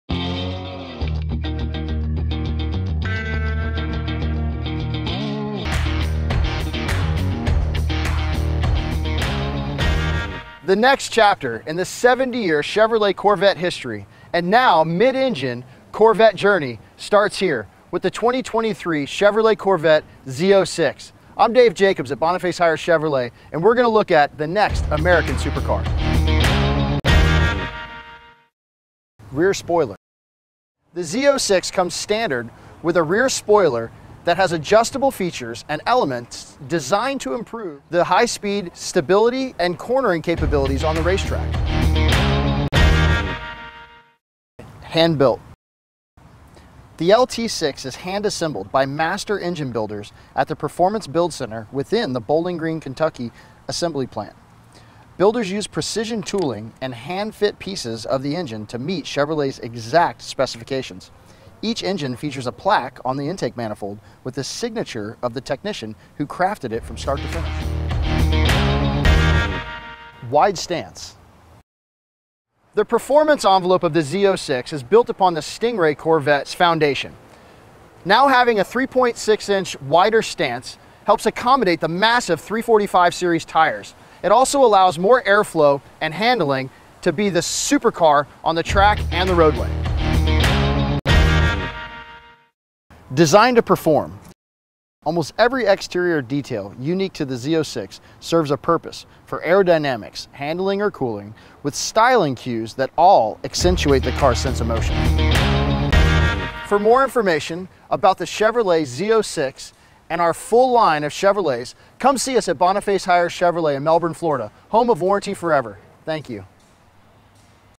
Matthew Jake Hiers - 2023 Z06, Spoiler, Hand built, Stance, Design, Melbourne, FL
Matthew Jake Hiers, 2023 Chevrolet Z06 Corvette, The Zo6 comes standard with an adjustable rear spoiler , The LT6 is hand assembled by master engine builders. , The performance envelope on the Z06 is built upon the Stingray Corvette’s foundation. Now having a 3.6” wider stance., Almost every exterior detail unique to the Z06 serves a purpose, for aerodynamics, handling or cooling., Matthew Jacob Hiers, Melbourne FL